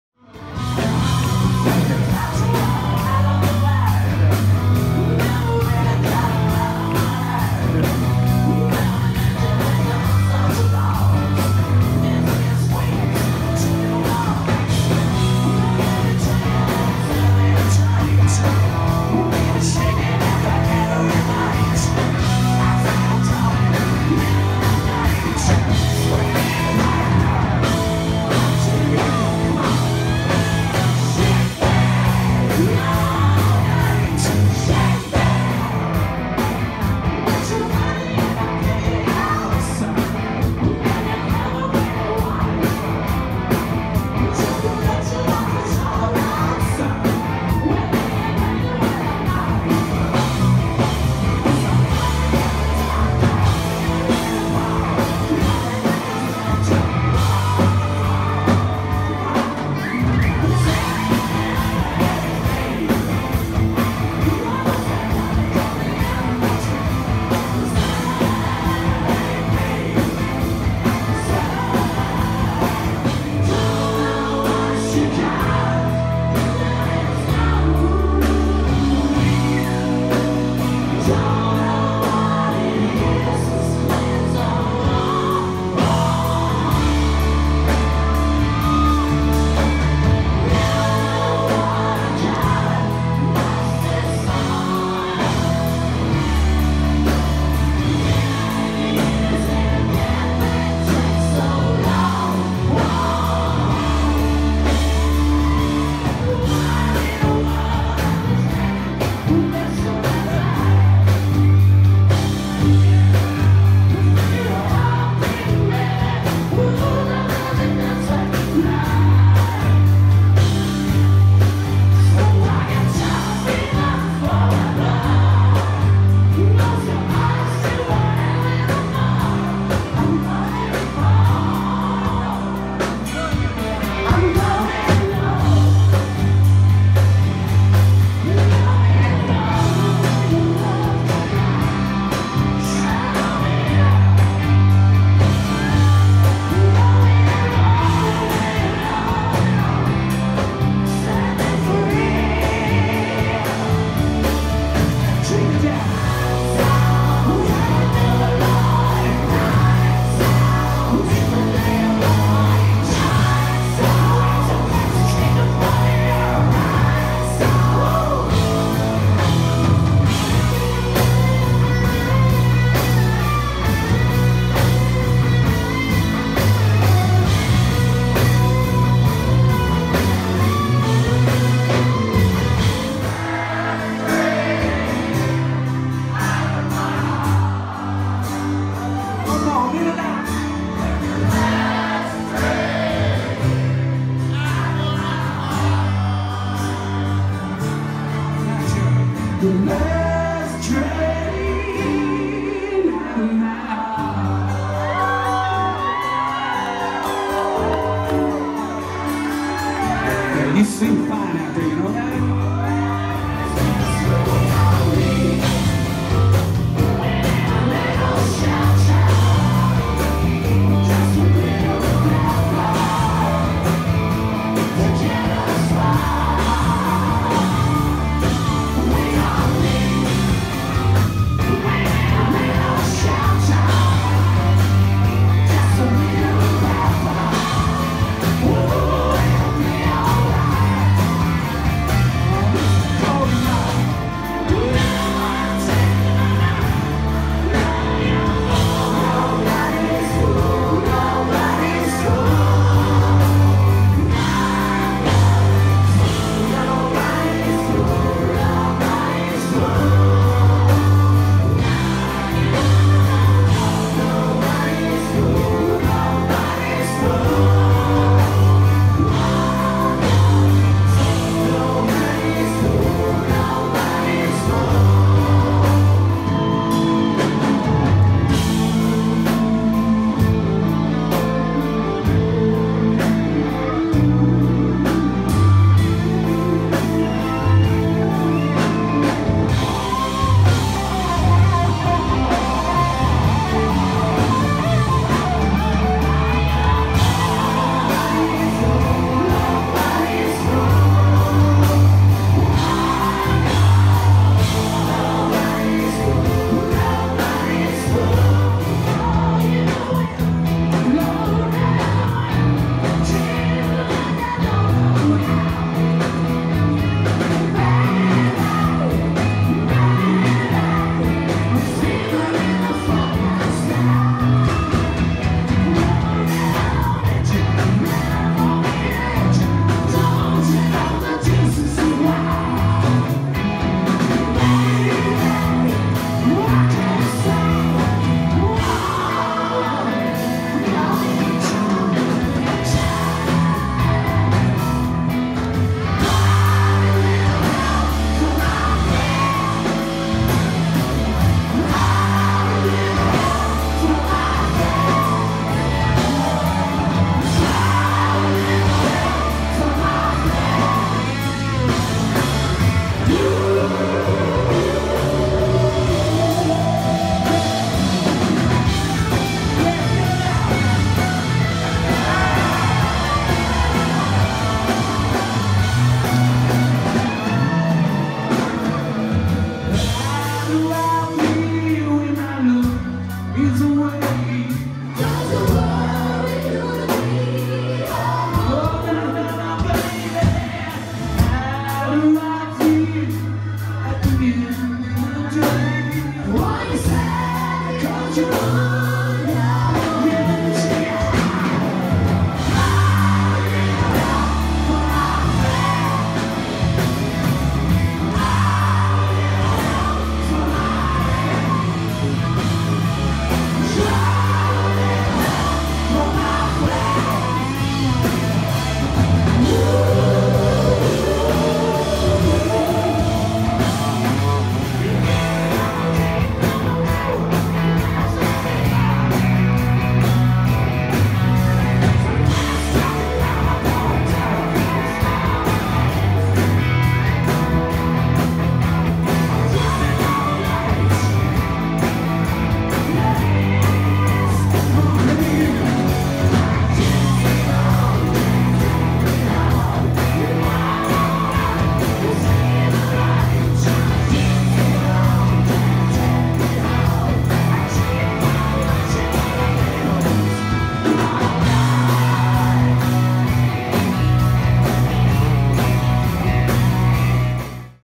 Tom Keifer @ The Cave
Tom Keifer of Cinderella performs with his band at the Cave in Big Bear Lake, Calif. on Nov. 11, 2017. Here are highlights from the evening.